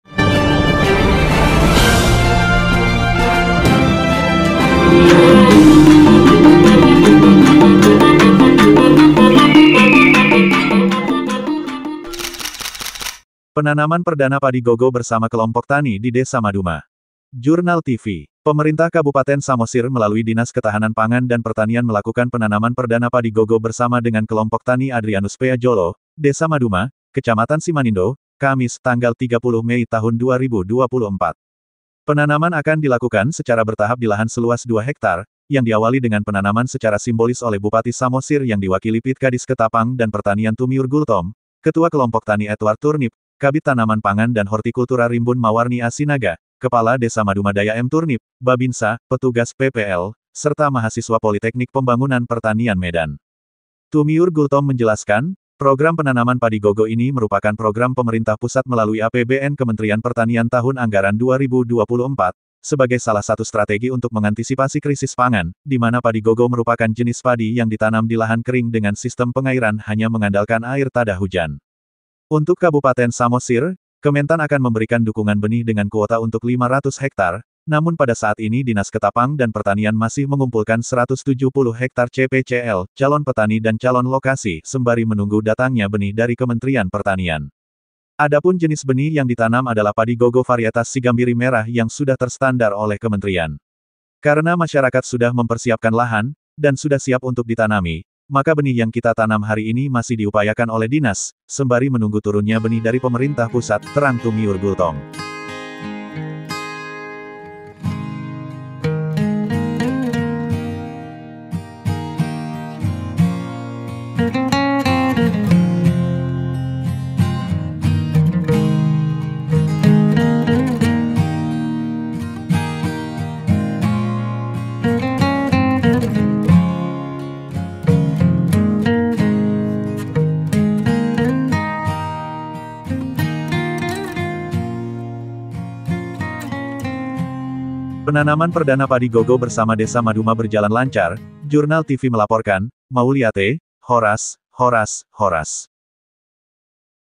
Penanaman Perdana Padi Gogo Bersama Kelompok tani di Desa Maduma
Jurnal tivi. Pemerintah Kabupaten Samosir melalui Dinas Ketahanan Pangan dan Pertanian melakukan penanaman perdana Padi Gogo bersama dengan Kelompok Tani  Adrianus Pea jolo, Desa Maduma, Kecamatan Simanindo, Kamis (30/5/2024).

Penanaman akan dilakukan secara bertahap dilahan seluas 2 Hektar, yang diawali dengan penanaman secara simbolis oleh Bupati Samosir yang diwakili Plt Kadis Ketapang dan Pertanian Tumiur Gultom, Ketua Kelompok Tani Edward Turnip, Kabid Tanaman Pangan dan Hortikultura Rimbun Mawarni A  Sinaga, Kepala Desa Maduma Daya M Turnip, Babinsa, petugas PPL, serta mahasiswa Politeknik Pembangunan Pertanian Medan.

Tumiur Gultom menjelaskan, program penanaman Padi Gogo ini merupakan program pemerintah pusat melalui APBN Kementerian Pertanian Tahun anggaran 2024, sebagai salah satu strategi untuk mengantisipasi krisis pangan, dimana Padi Gogo merupakan   jenis padi yang ditanam dilahan kering dengan sistem pengairan hanya mengandalkan air tadah hujan.

Untuk Kabupaten Samosir, Kementan akan memberikan dukungan benih dengan kuota untuk 500 Hektar, namun pada saat ini Dinas Ketapang dan Pertanian masih mengumpulkan 170 Hektar  CPCL (Calon Petani dan Calon Lokasi), sembari menunggu datangnya benih dari Kementerian Pertanian.

Adapun jenis benih yang ditanam adalah Padi Gogo varietas Sigambiri Merah yang sudah terstandar oleh Kementerian.

"Karena masyarakat sudah mempersiapkan lahan, dan sudah siap untuk ditanami, maka benih yang kita tanam hari ini masih diupayakan oleh Dinas, sembari menunggu turunnya benih dari pemerintah pusat", terang Tumiur Gultom.

Penanaman Perdana Padi Gogo Bersama Desa Maduma berjalan lancar,Jurnal Tivi melaporkan,Mauliate,Horas,Horas,Horas.